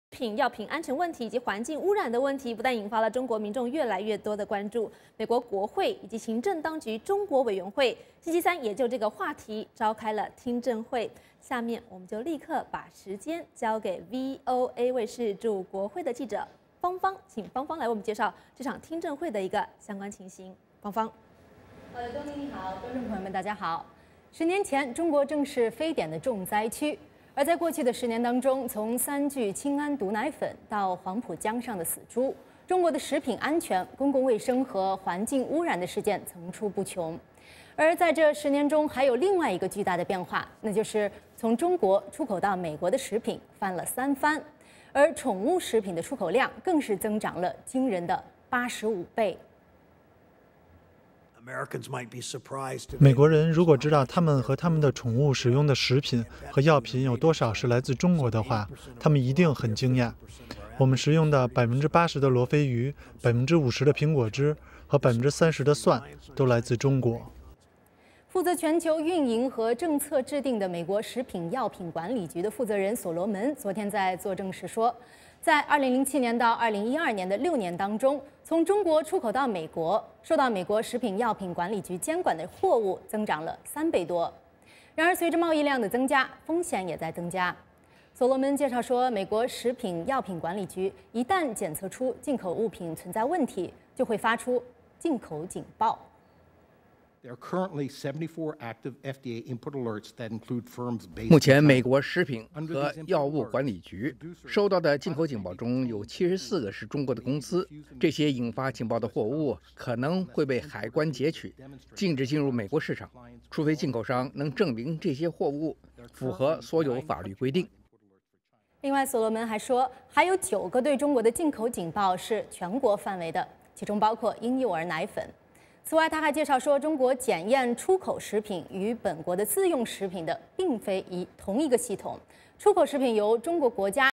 美议员：中国无法解决食品安全问题
中国的食品药品安全问题以及环境污染问题不仅引发中国民众越来越多的关注，美国国会及行政当局中国委员会22号也就这个话题召开了听证。脸书: